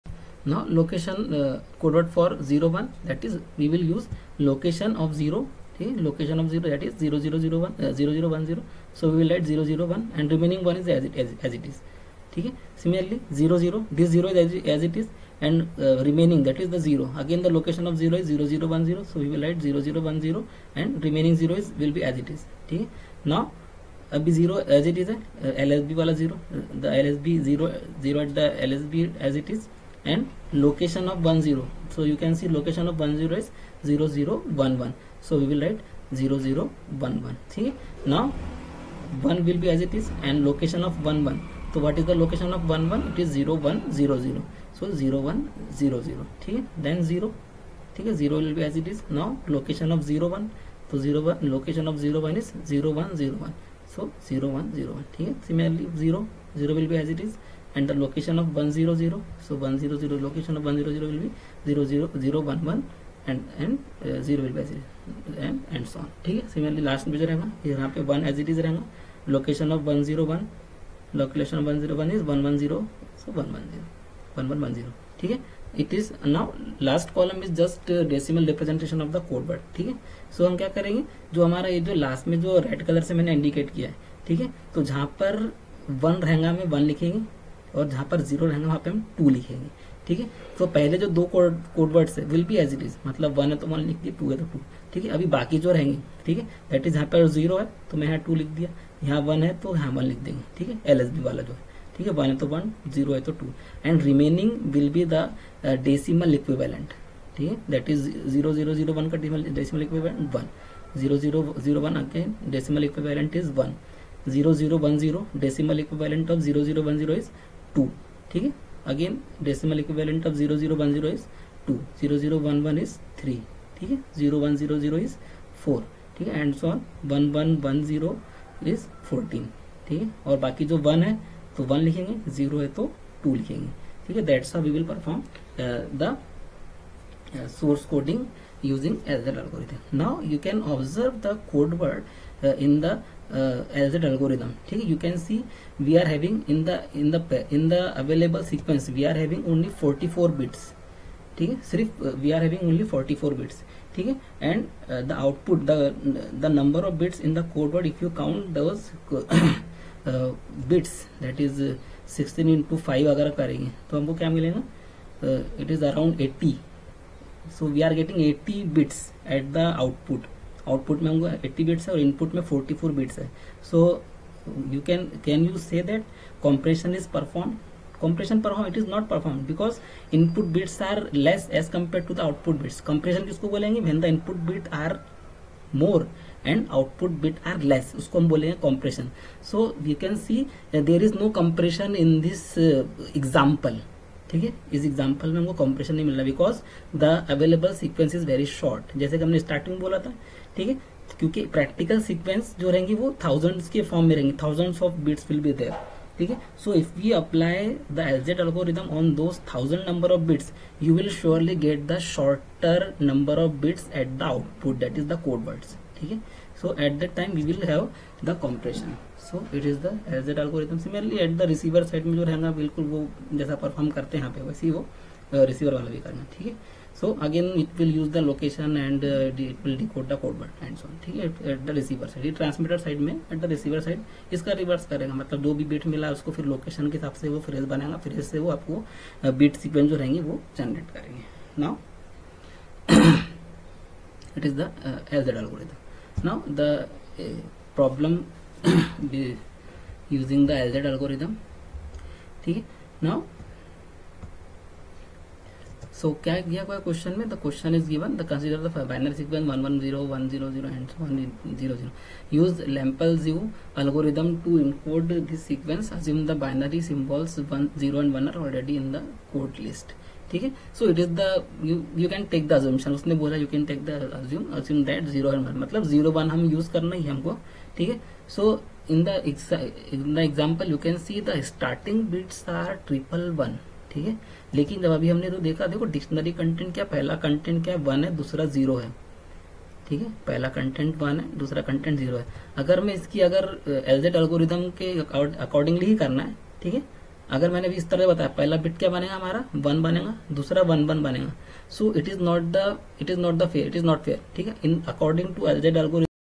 Digital Communication lecture 2.3.3
Lempel-Ziv Algorithm (LZ Algorithm):
L-Z source coding algorithm is design to be independent of the source statistics. Hence, the source coding algorithm belongs to the class of “Universal source coding algorithms”. It is variable to fixed length algorithm